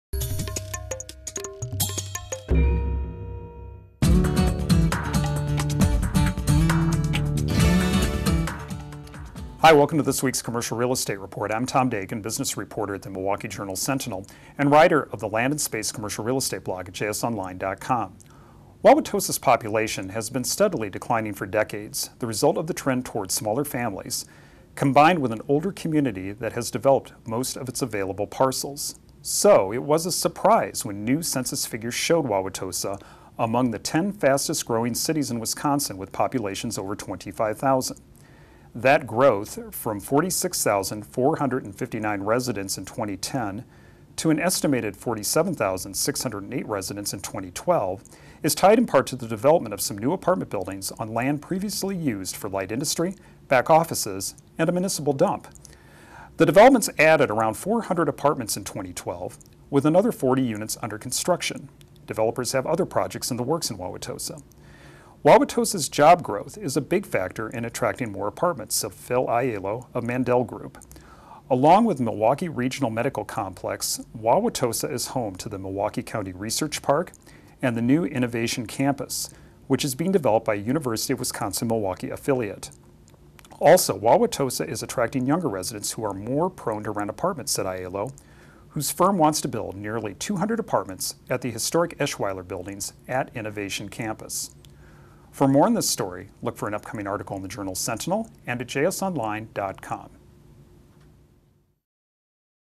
Land and Space: Growth in Wauwatosa
Wauwatosa’s population has been steadily declining for decades. So it was surprise when the Census Bureau recently released figures that showed Wauwatosa among the 10 fastest growing cities in Wisconsin with populations over 25,000.